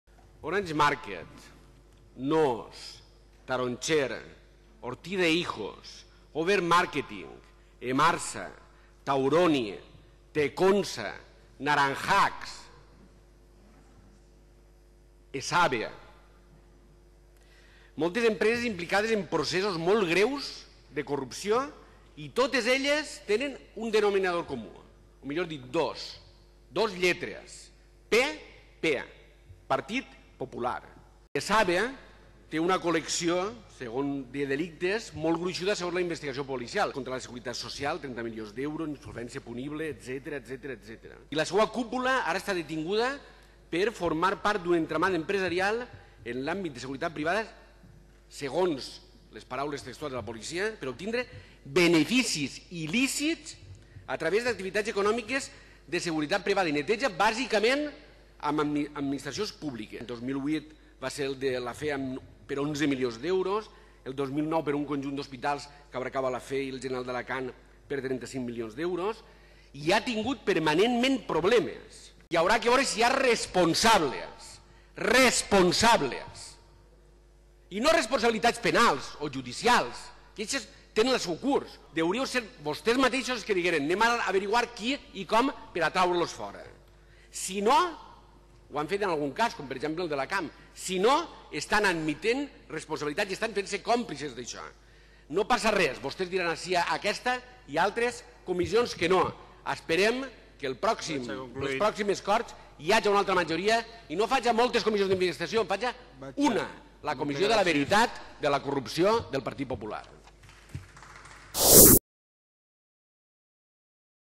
El PP col·lecciona corrupció. ESABE: contractes públics a canvi de comissions.
Orange Market, Nóos, Tauroni, Emarsa, Over Marketing, Naranjax, Ortiz e Hijos... Com si només fóra una col·lecció d'estampetes, el PP amplia la seua nòmina corrupta amb ESABE i el presumpte pagament de comissions al grup empresarial per part de la Generalitat a canvi de contractes públics. I, una altra vegada, la majoria absoluta popular va aconseguir, en Les Corts, vetar l'obertura d'una comissió d'investigació sobre el cas.

Ple de les Corts Valencianes, 24 de juliol de 2013.